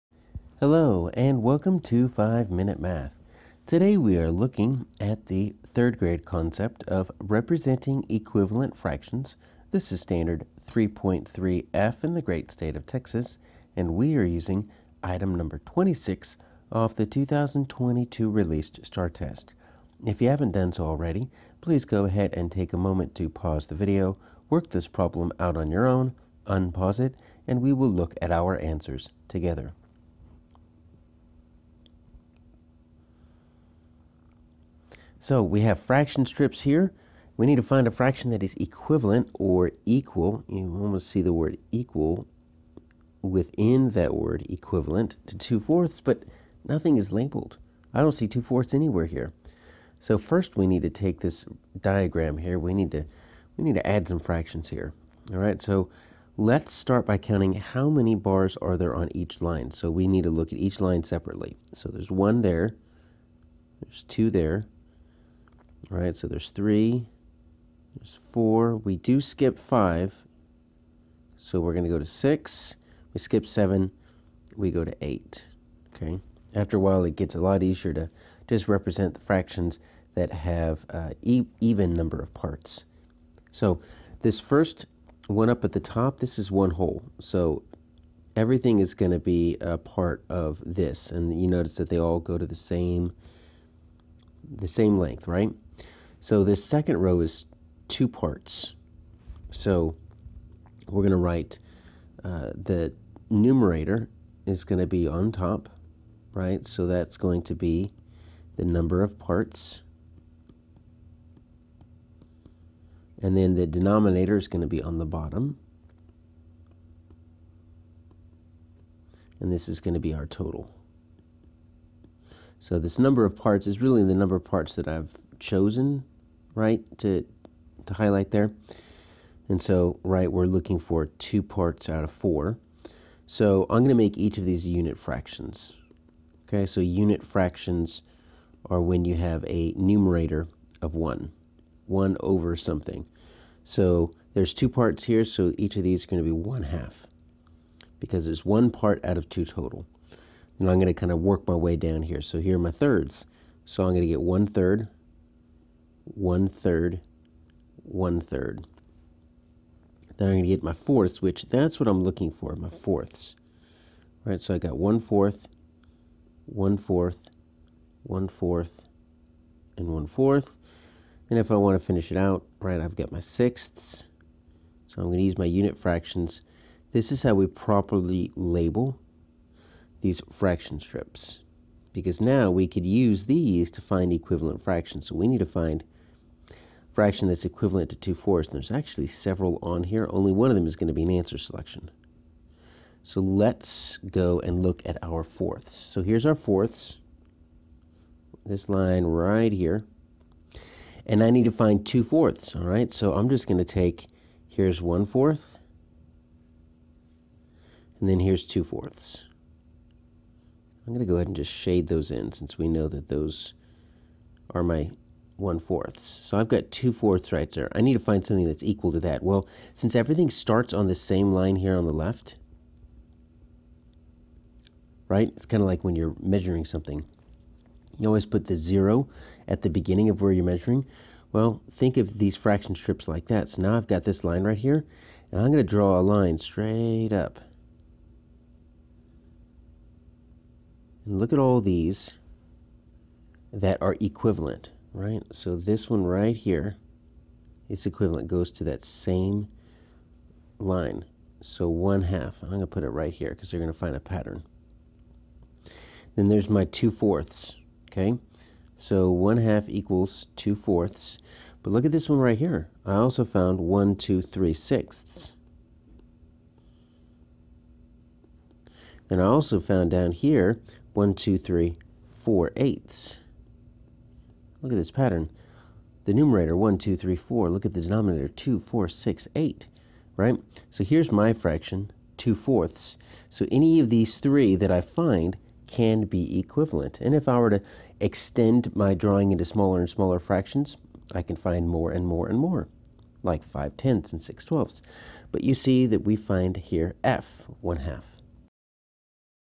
3rd Grade STAAR Practice Representing Equivalent Fractions (3.3F - #7)
Practice representing equivalent fractions with denominators of 2, 3, 4, 6, and 8 using a variety of objects and pictorial models, including number lines, with a test item from the 2022 STAAR test (#26 - 3.3F).